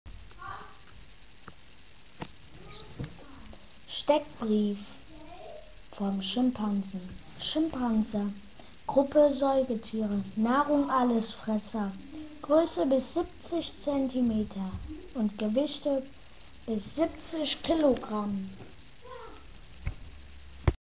Steckbrief: Schimpanse
Gruppe:Säugetiere
Nährung:Allesfresser
Größe:bis 170 cm
Gewicht:bis 70 kg